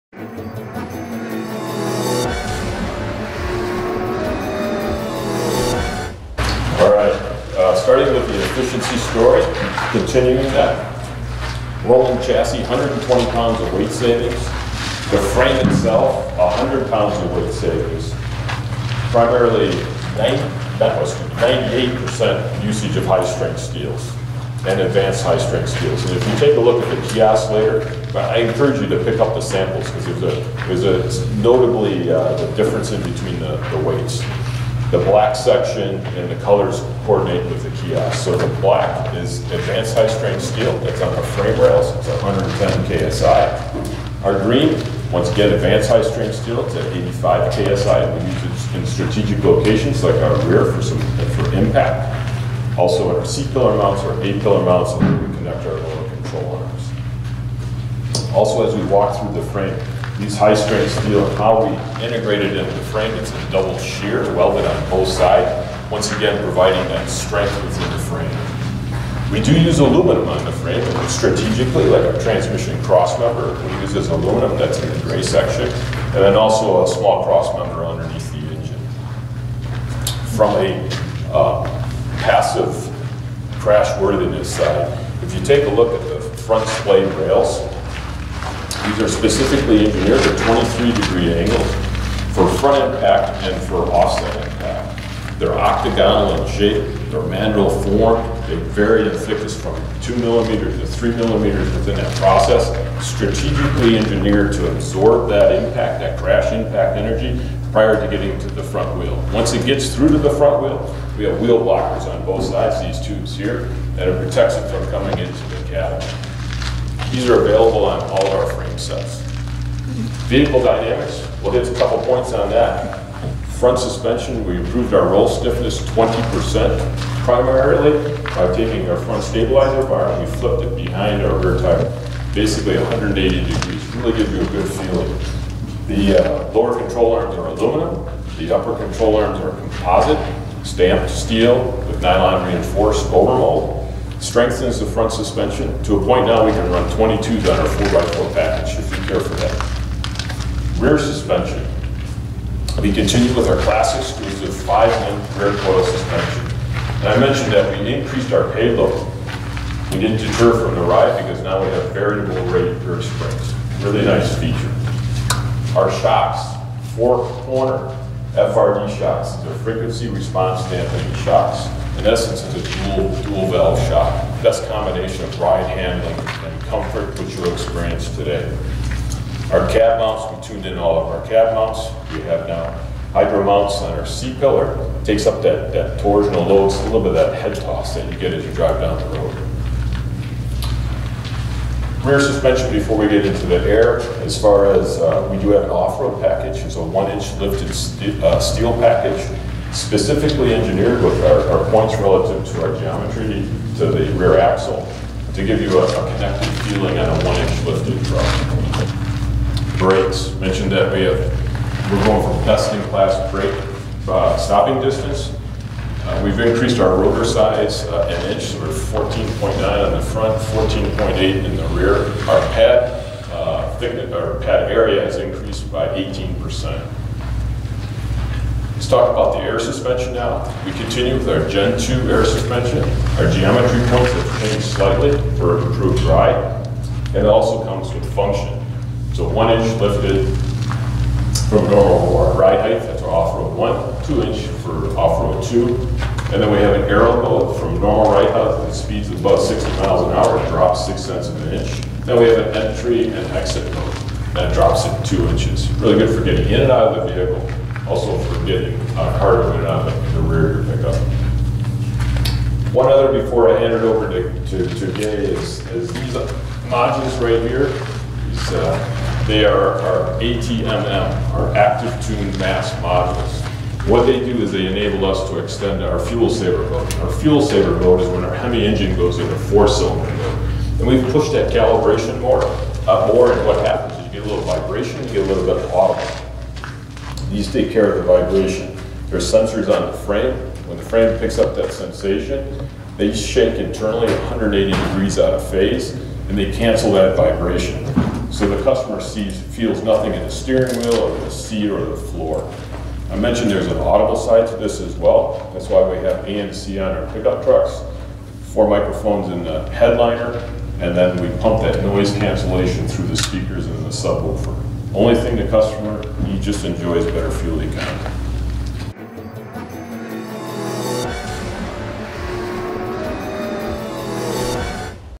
2019 RAM 1500 Technical Presentation by Rob Whichman, Head of RAM Engineering
2019 RAM 1500 Technical Presentation by Rob Whichman, Head of RAM Engineering.

The all-new 2019 Ram 1500 becomes the benchmark for durability, technology, efficiency and convenience with features never before offered in a pickup.

Overall weight for the Ram 1500 has been reduced by 225 pounds. As the truck’s backbone, the frame uses advanced materials and engineering to eliminate 100 pounds while increasing stiffness and durability for 12,750 pounds of towing capability and 2,300 pounds of payload.

An all-new eTorque system delivers improved fuel efficiency in both V-6 and V-8 configurations.

As a segment disrupter, the new Uconnect 4C with a massive 12-inch touchscreen leads the pickup world with technology featuring split-screen capability, 360-degree camera views and exclusive content from SiriusXM with 360L.

Active safety and security systems join the technology onslaught with adaptive cruise control, forward collision warning, Blind-spot Monitoring and ready alert braking.